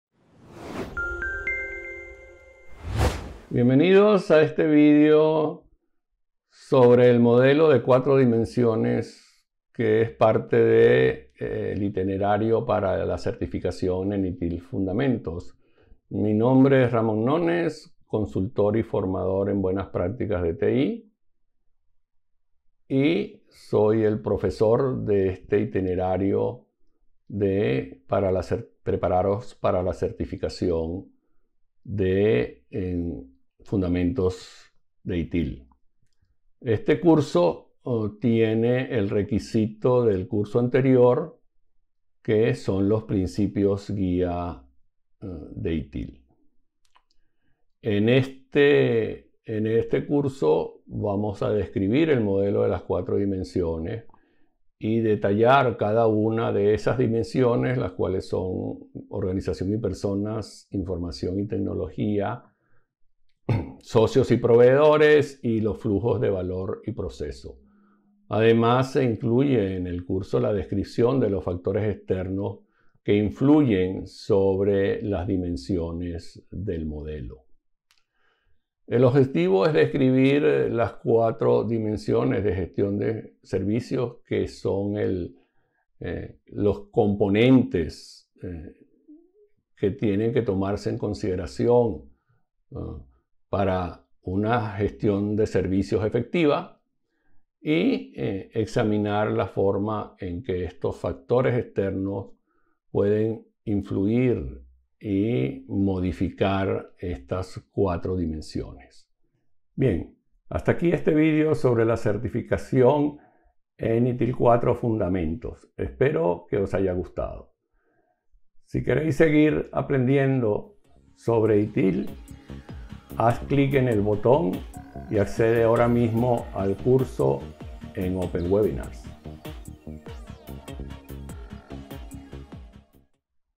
Curso ITIL 4 Fundamentos Parte 3: Las 4 Dimensiones de la Gestión de Servicios
Continuamos con este tercer curso del itinerario dedicado a la certificación de ITIL 4, en el que aprenderás a describir el modelo de las 4 dimensiones de la gestión de servicios, y cómo apoyan a la creación de productos y servicios.

- Entender el modelo de 4 Dimensiones de ITIL 4
- Comprender cómo cada una de las 4 dimensiones contribuyen a la creación y entrega de los - productos y servicios
- Identificar los conceptos, herramientas y procesos correspondientes a cada una de las dimensiones
- Entender los elementos externos que condicionan las 4 dimensiones

Comienza este Curso ITIL 4 Fundamentos Parte 3: Las 4 Dimensiones de la Gestión de Servicios